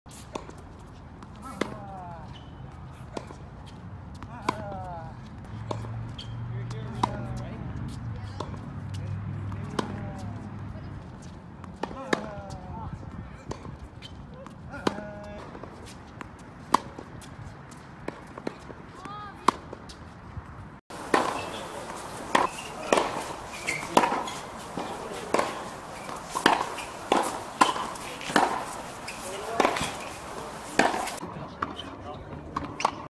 테니스 백핸드 스트로크 이형택
TENNIS COLLECTION!

백핸드 스트로크를 이렇게 사정없이 치고 싶다.
이형택처럼 이렇게 강력한 백핸드를 치고 싶다.